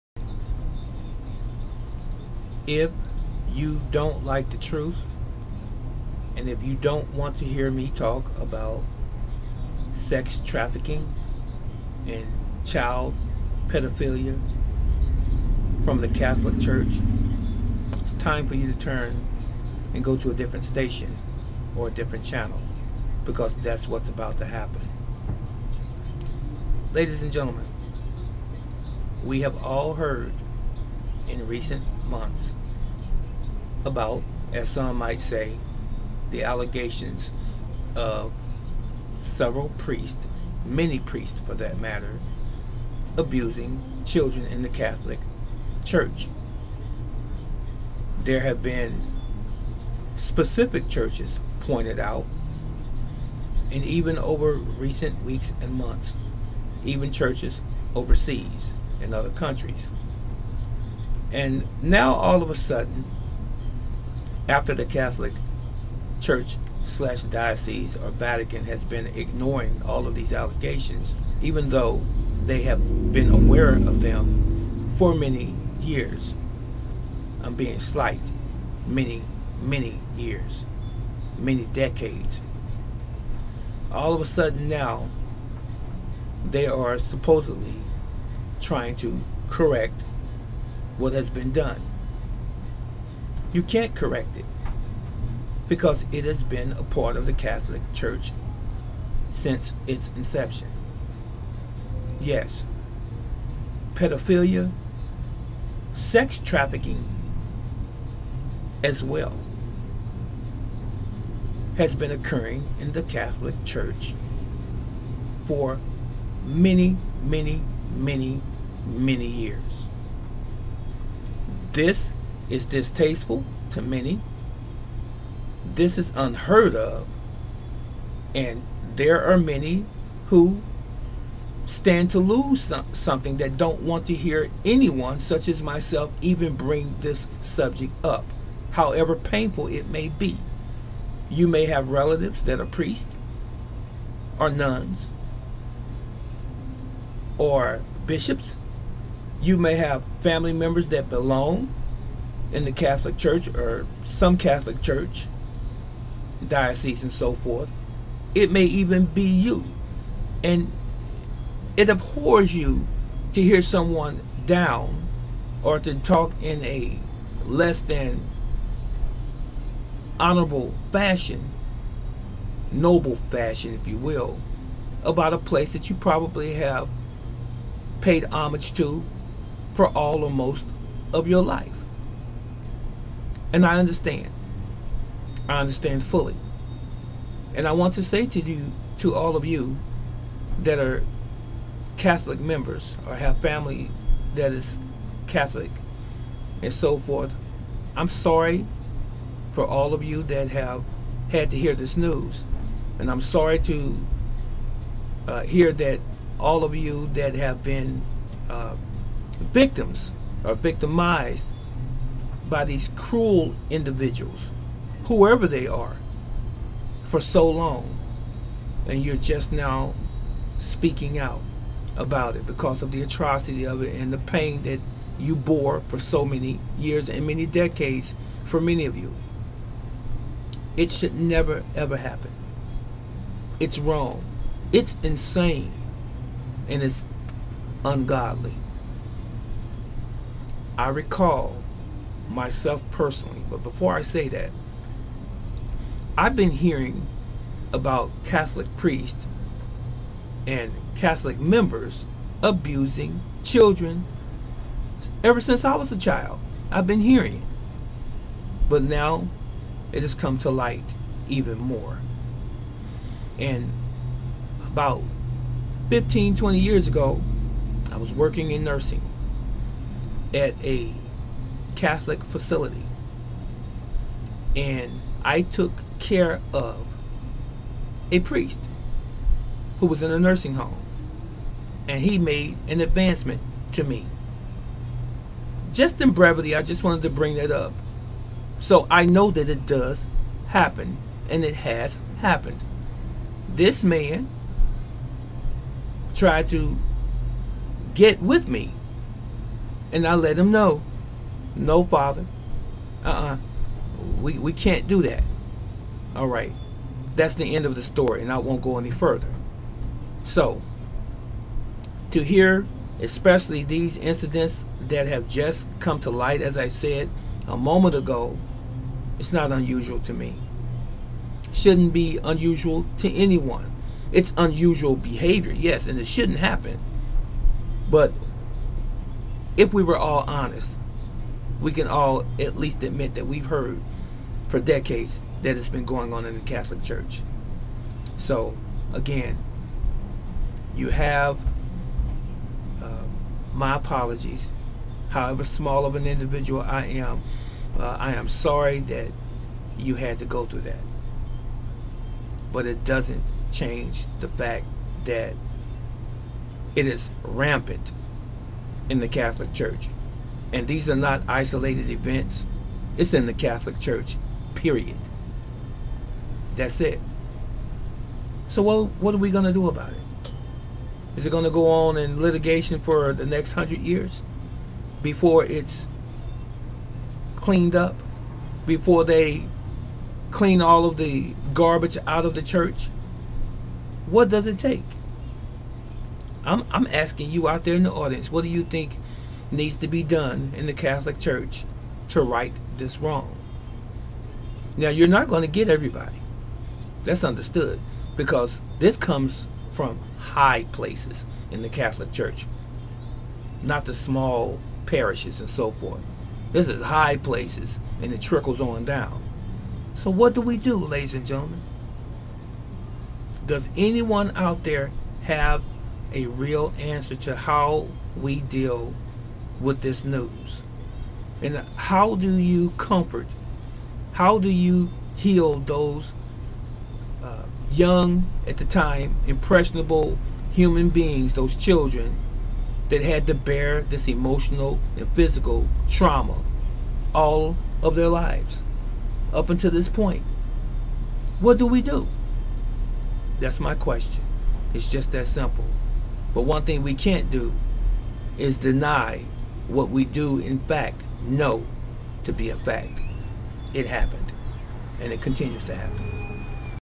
How do we fix Pedophilia in the Catholic church?
Most of us know about the recent, and even long time allegations of sexual misconduct in the Catholic church, but what next.
As much as we would like to dismiss it, or even deny it, it is real, and must be addressed immediately.
There is too much information, case evidence and priest that have admitted to this behavior being true, throughout the Catholic church. This is all public record..